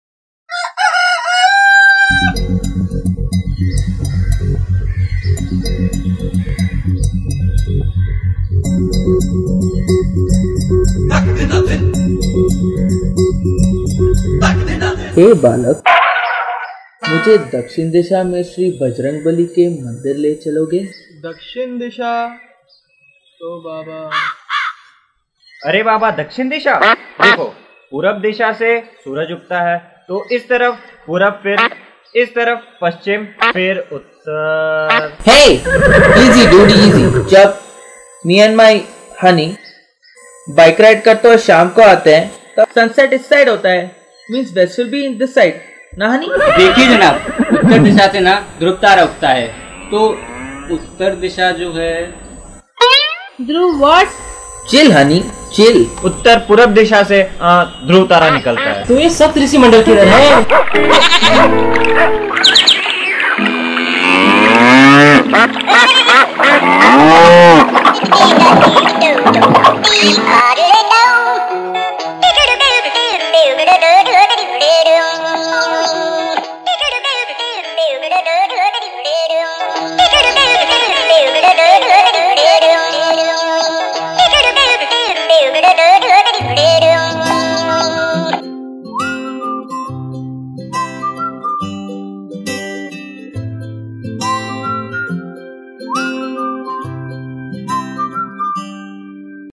GPS advertisement
My Directed and edited 1st advertisement on GPS system ... Plz Watch guys  and giv Your Suggestions and comments So do I improve As well. .njoy. :D :D :D